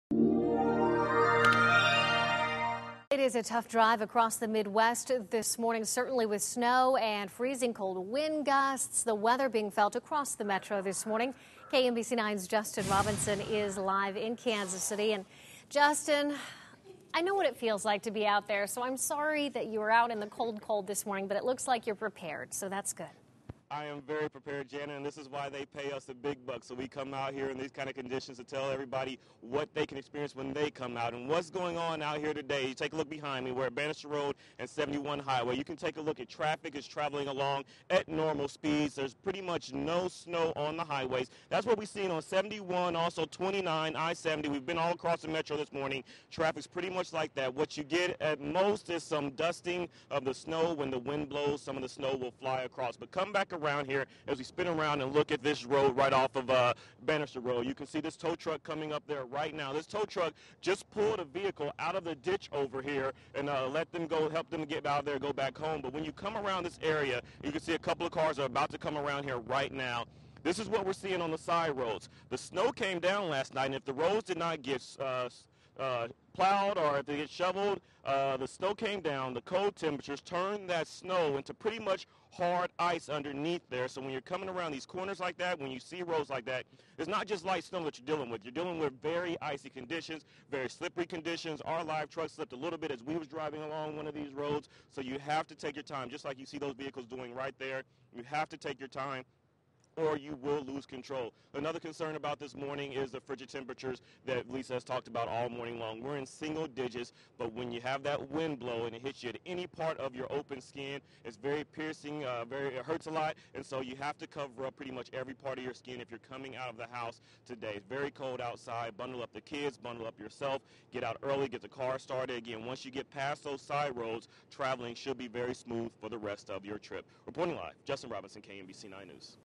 Side Streets May Be Messy
Most highways and main streets in the Kansas City area are clear, but drivers should be careful on snow and ice-packed side streets. KMBC 9's Justin Robinson reports.